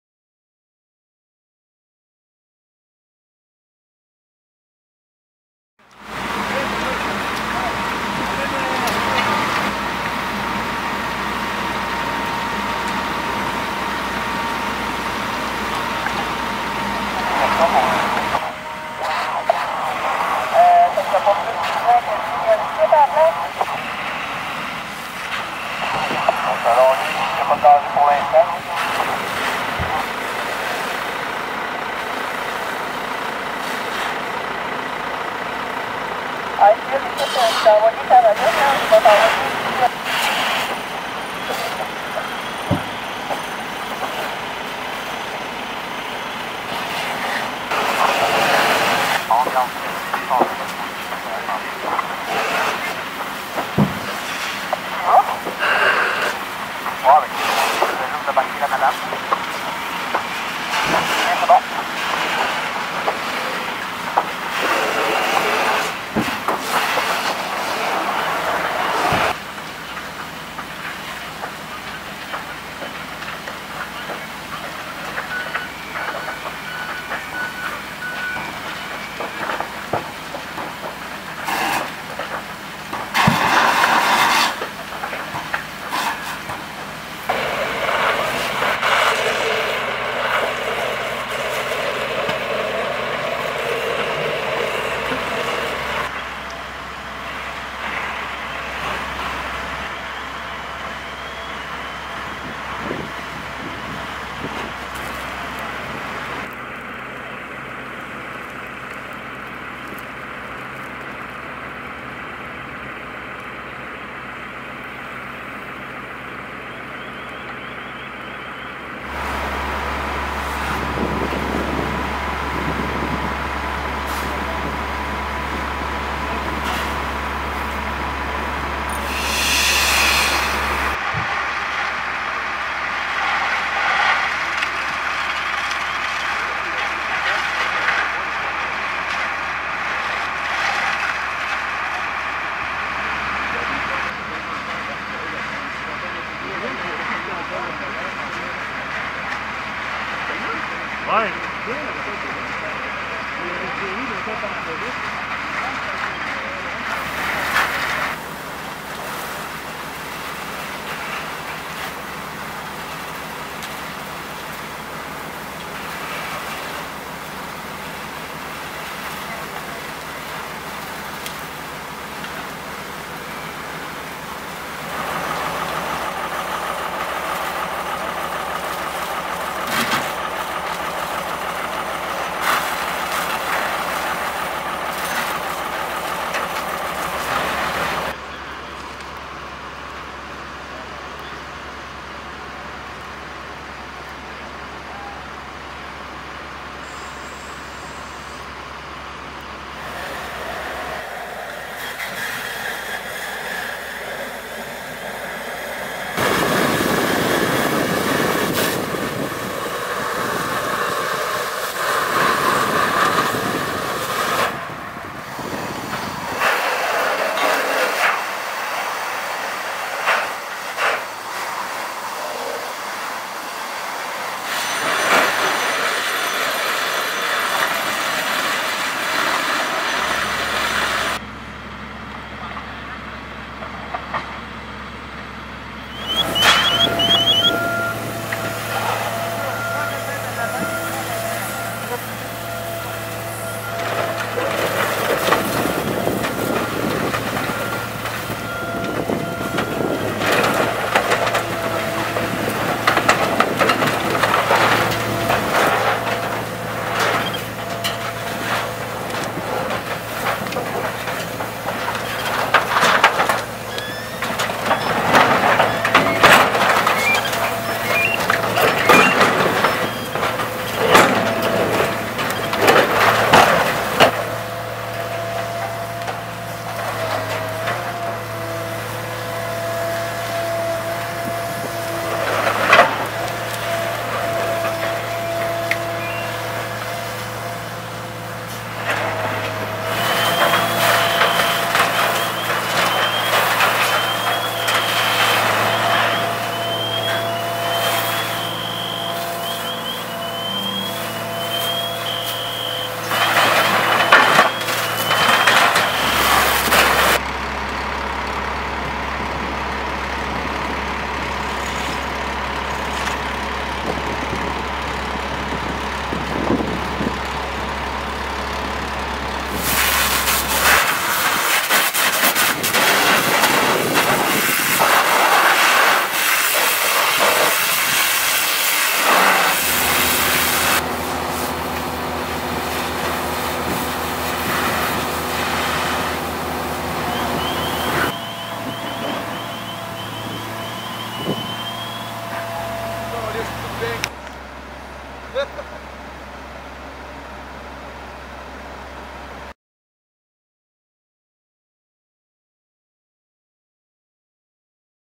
22 décembre 2016 - Incendie Legault Métal
Incendie des garages mécanique chez Legault Métal le 22 décembre 2016

Service des incendies d'Amos et quelques pompiers de Landrienne sont intervenus sur cet incendie